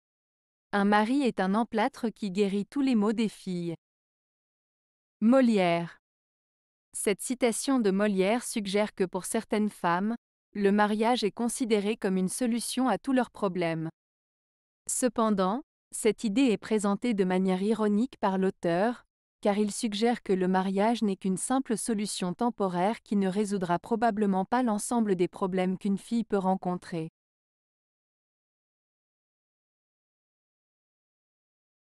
Un mari est un emplâtre qui guérit tous les maux des filles. - Molière
Cette citation de Molière suggère que pour certaines femmes, le mariage est considéré comme une solution à tous leurs problèmes. Cependant, cette idée est présentée de manière ironique par l'auteur, car il suggère que le mariage n'est qu'une simple solution temporaire qui ne résoudra probablement pas l'ensemble des problèmes qu'une fille peut rencontrer.